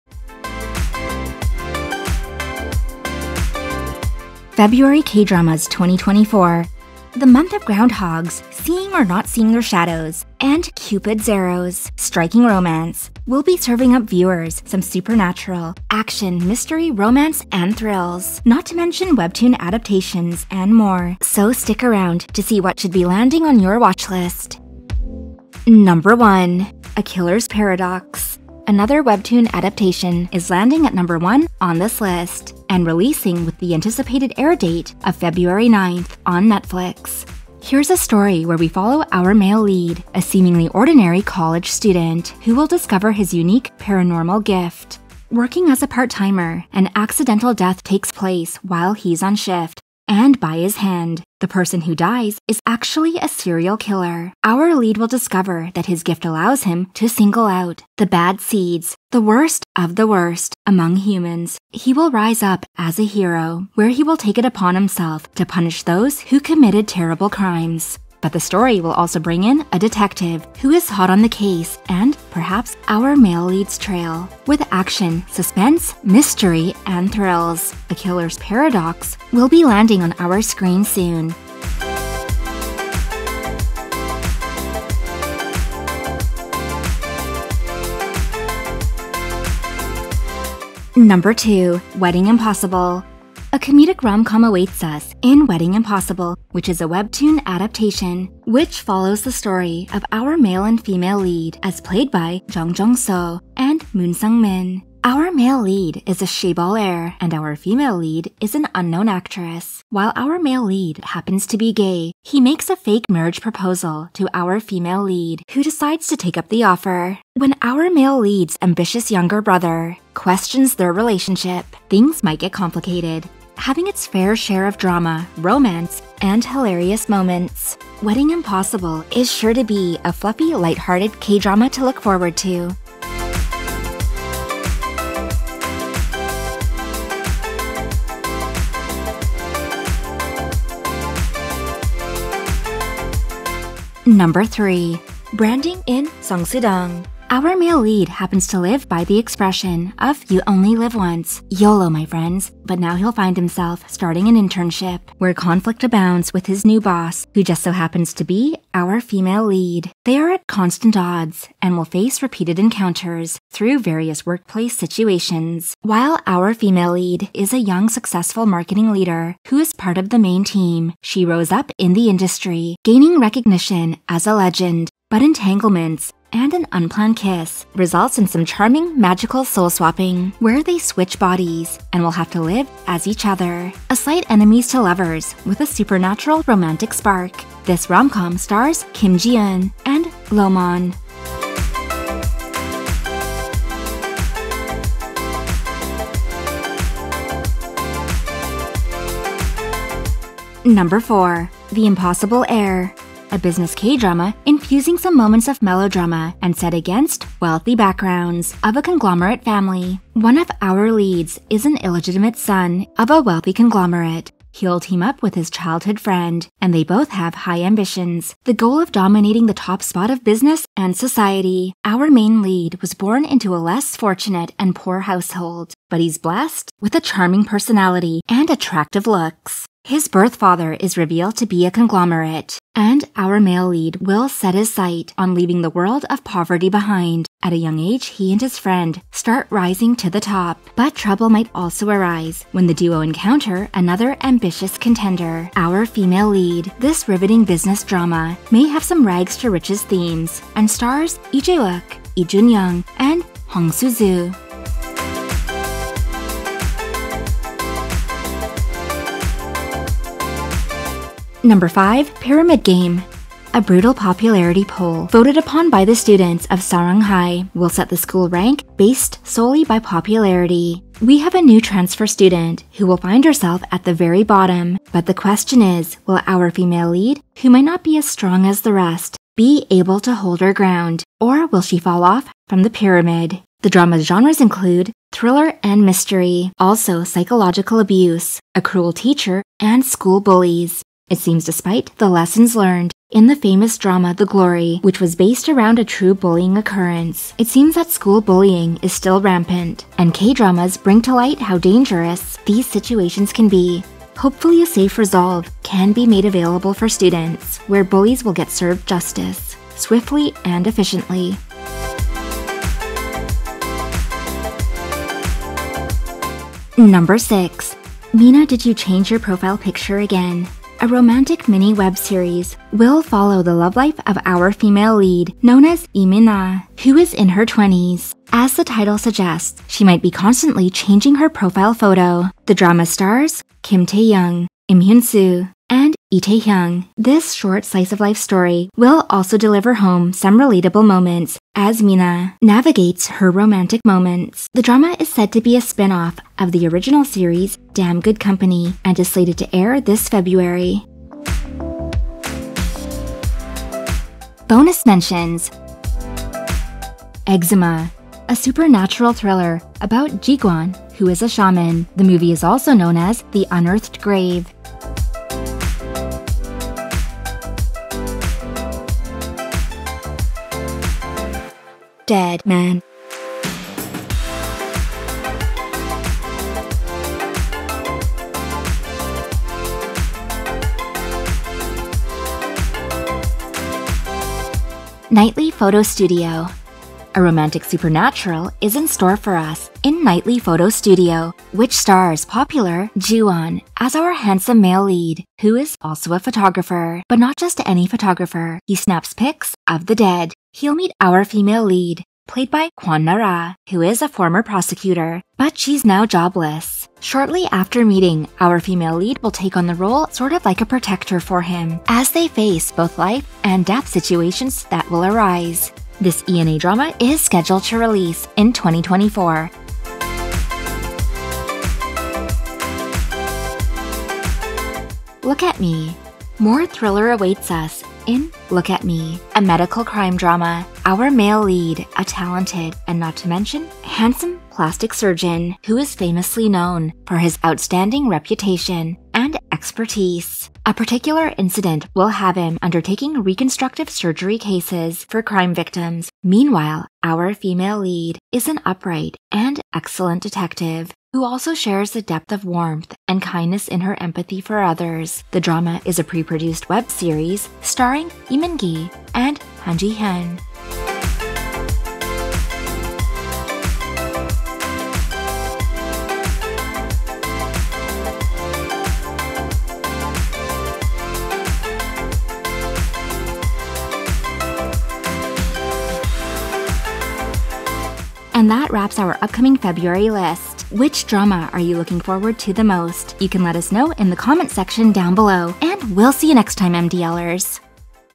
9 Hottest Korean Dramas To Watch in February 2024 [Ft. HappySqueak]
In this episode, we go over the 9 highly anticipated Kdramas airing this February 2024 on Netflix Viki Viu iqiyi and more! What Koreandramas airing this February are YOU excited about?